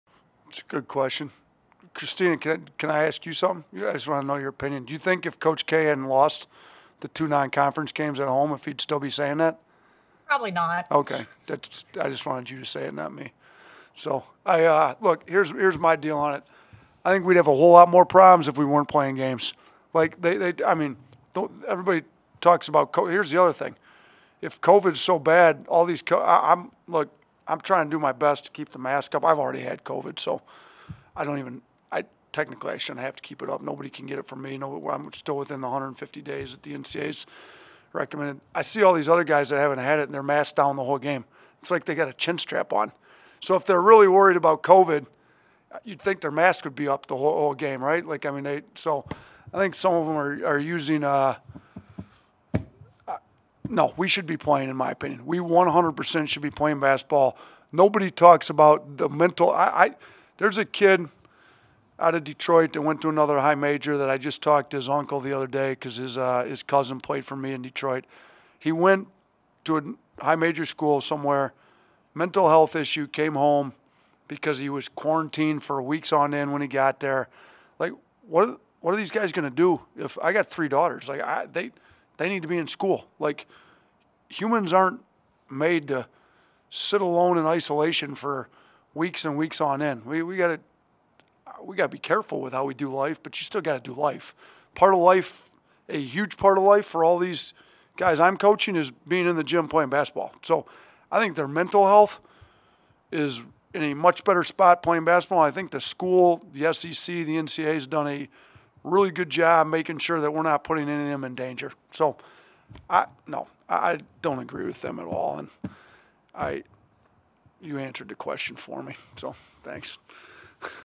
Nate Oats: Would Coach K be voicing COVID-19 concerns if he hadn't lost 2 games?
Hear what Alabama men's basketball coach Nate Oats had to say when asked about Duke coach Mike Krzyzewski recently voicing concerns about playing the season amid the COVID-19 pandemic following his team's second loss in non-conference play.